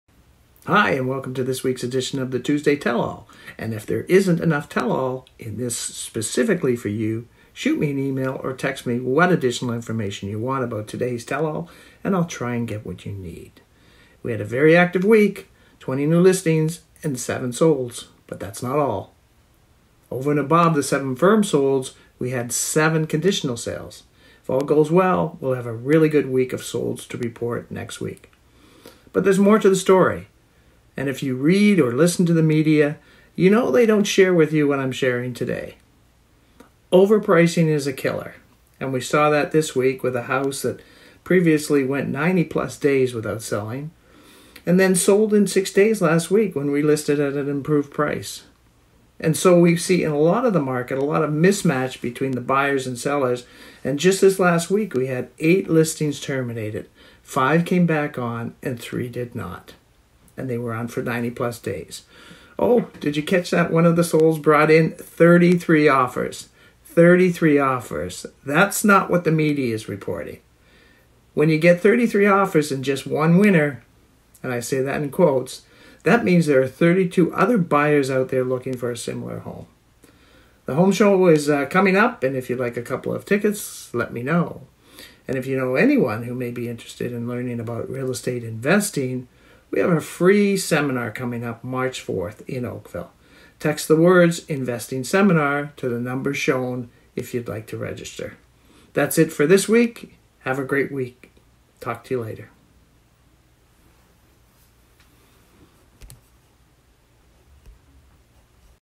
Oakville (Bronte) Real Estate Market Update Feb 13th 2023
Here is your Oakville (Bronte) real estate market update for the week ending Feb13th, 2023. This is your source of Information for the Bronte Real Estate Market